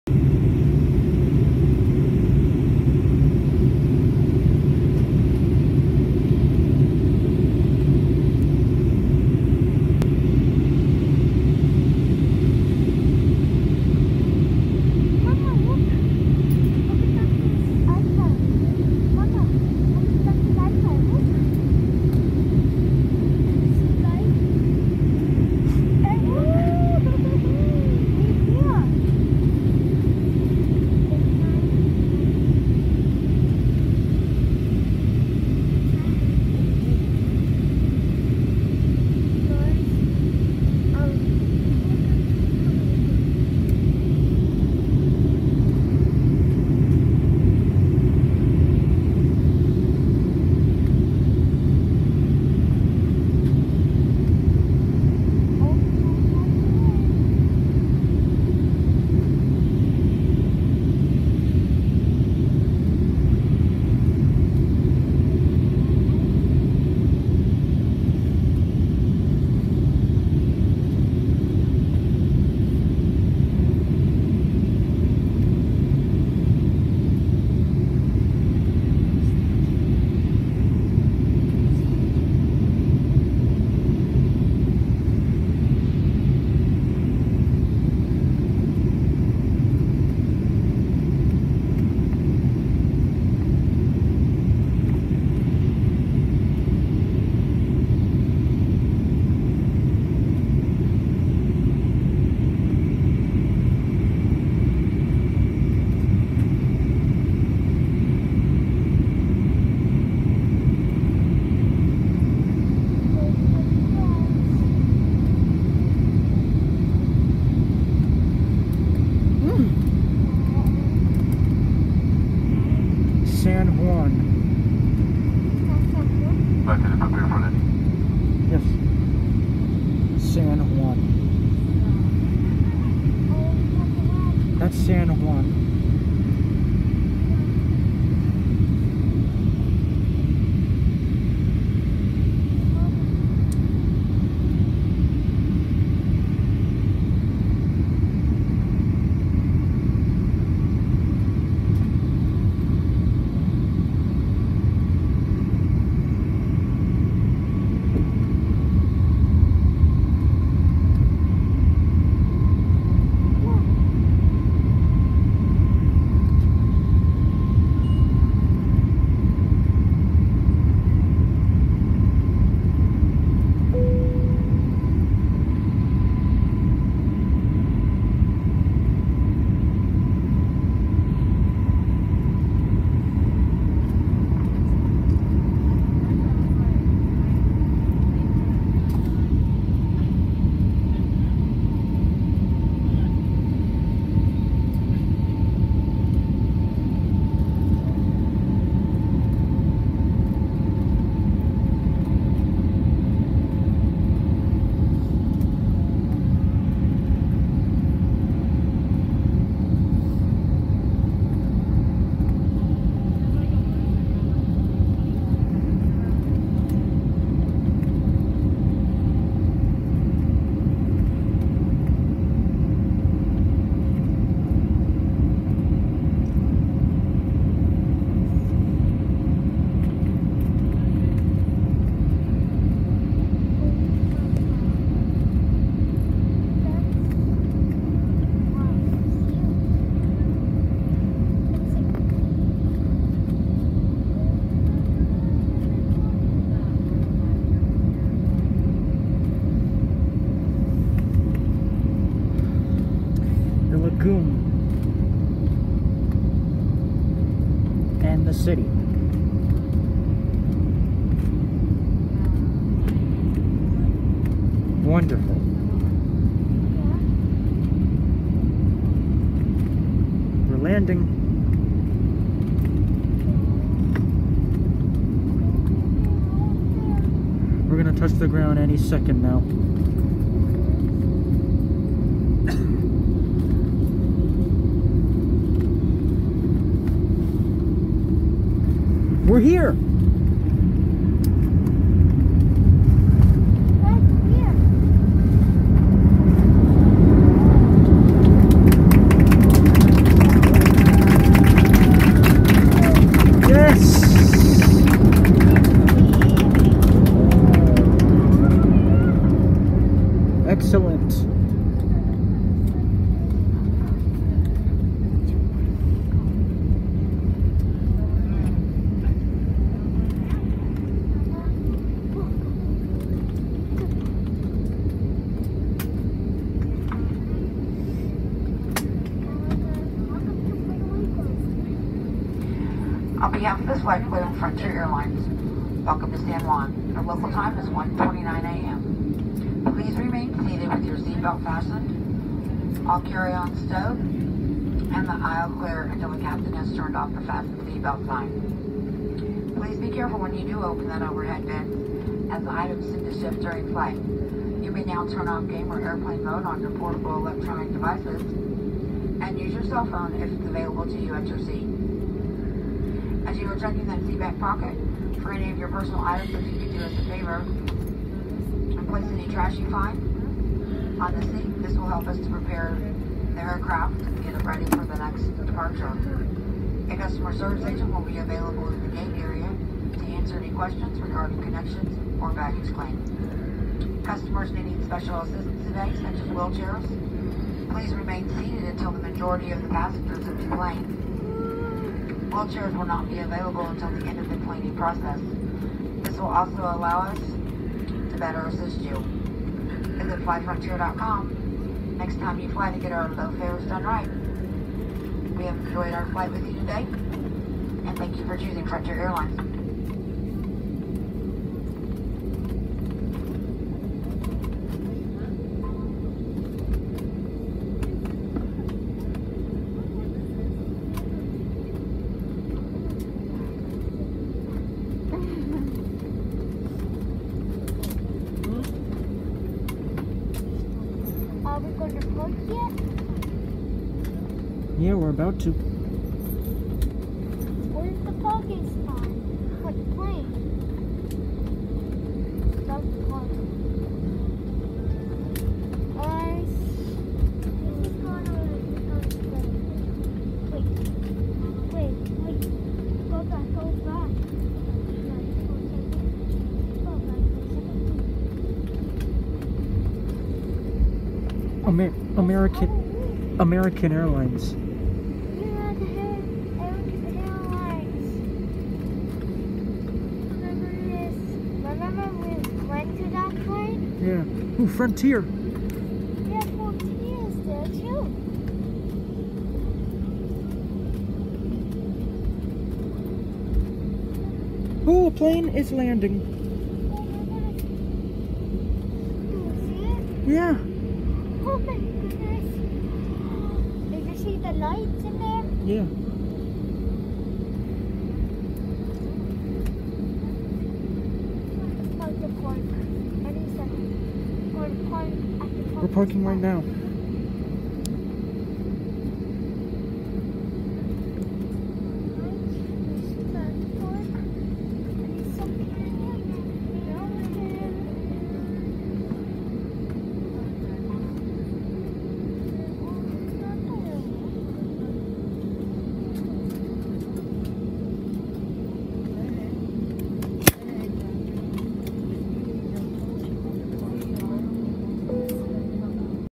Frontier Landing In SJU International Airport
We landed in Orlando, Florida, waited for a few hours for the next plane ✈️, we went in the next plane ✈️, took off from Orlando, Florida, and landed in SJU International Airport.